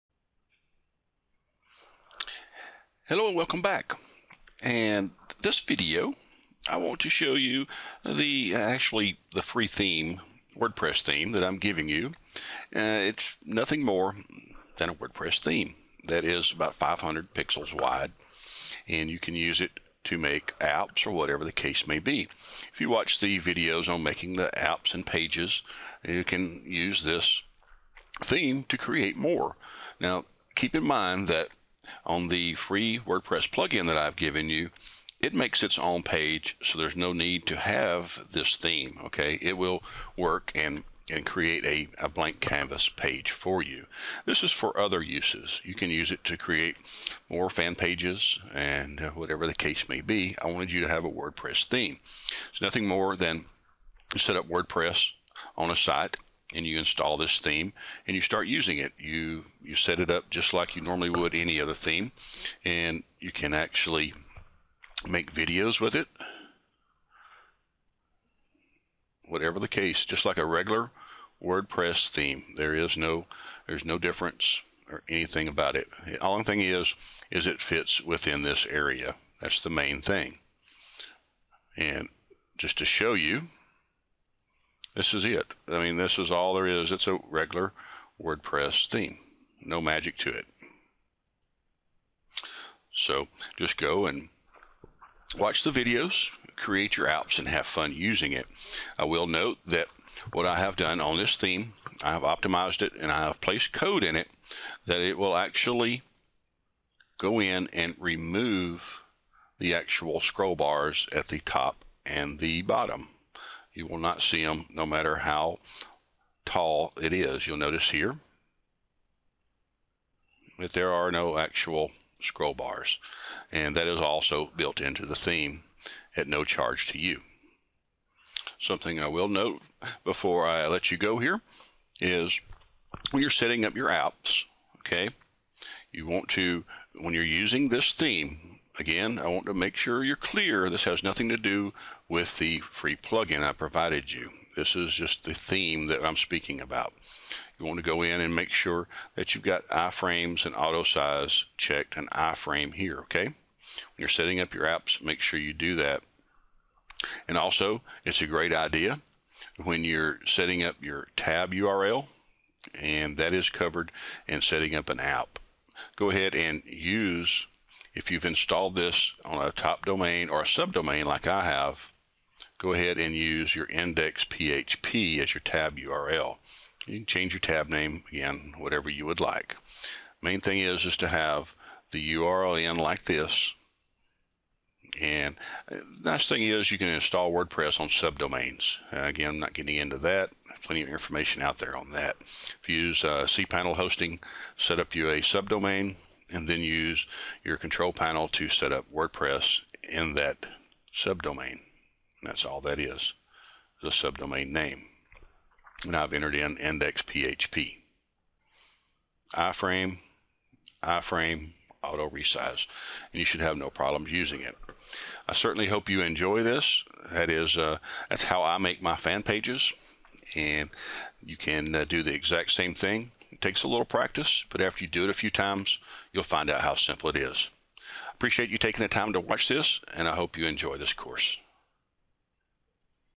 Free Wordpress Theme Optimized For Facebook Pages and Applications
- Use my free Wordpress theme optimized for the new Facebook iframes. Great for Facebook Fanpages or Facebook Applications.